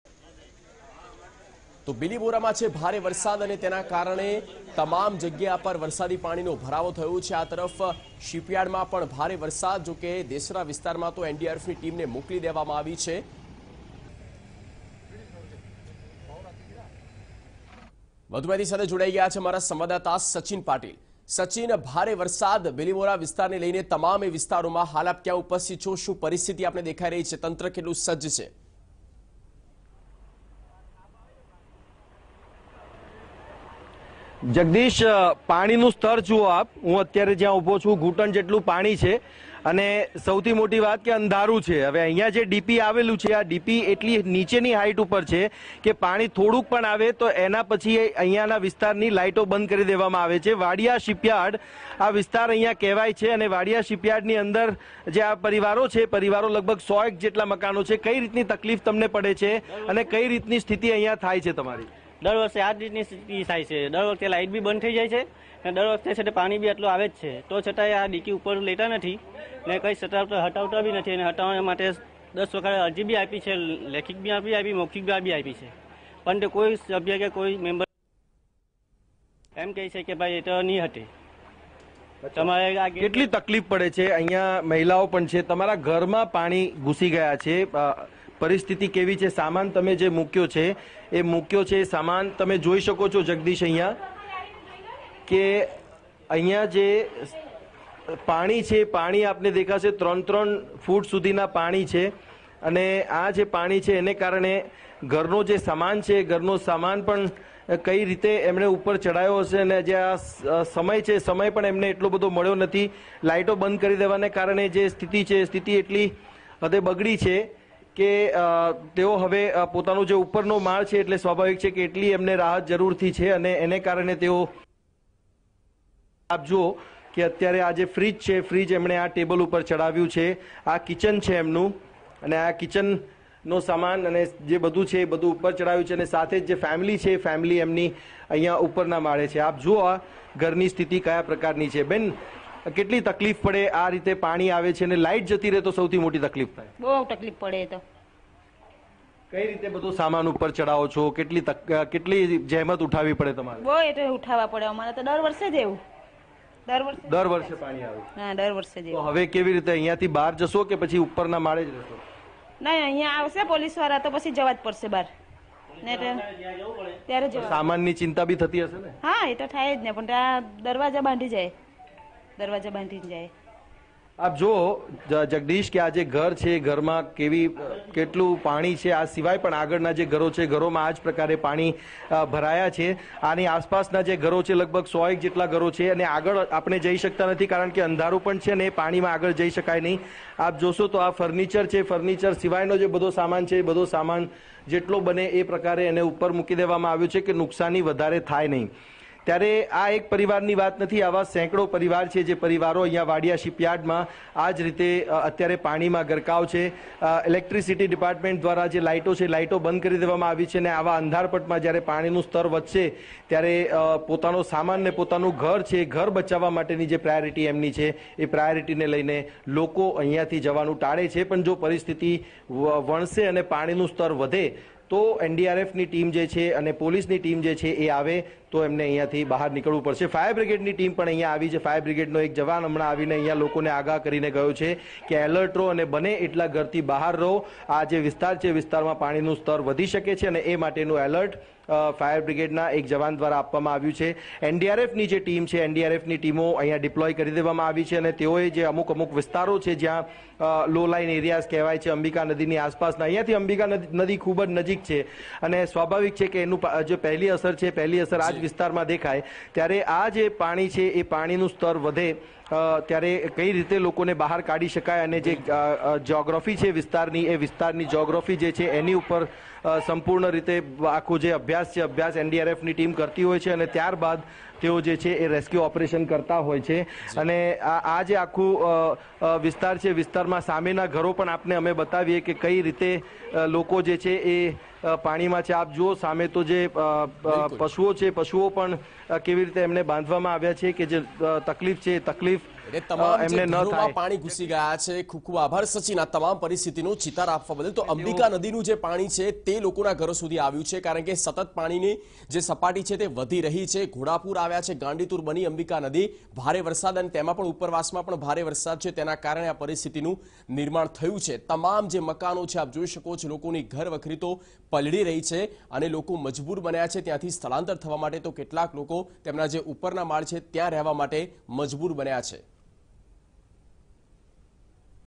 Heavy rain in Bilimora, water enters houses | Navsari - Tv9GujaratiNews
Heavy rain in Bilimora, water enters houses | Navsari